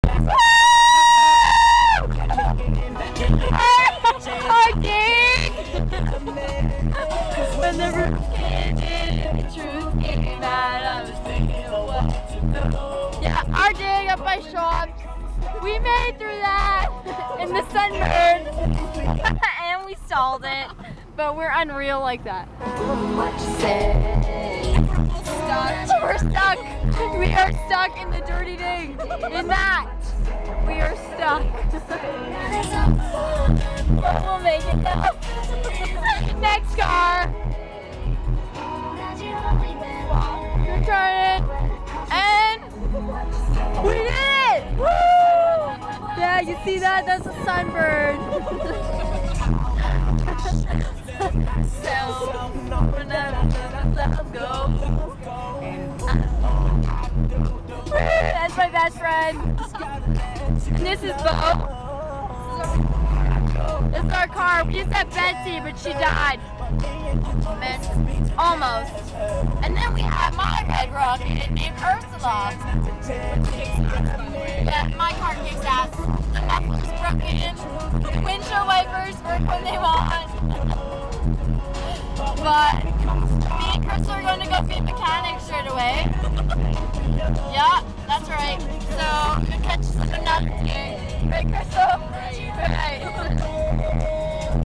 Crystal and Laken Drifting 001
Me and Laken thought it would be fun to pull my e-brake in my Sunbird, and it was a blast. lol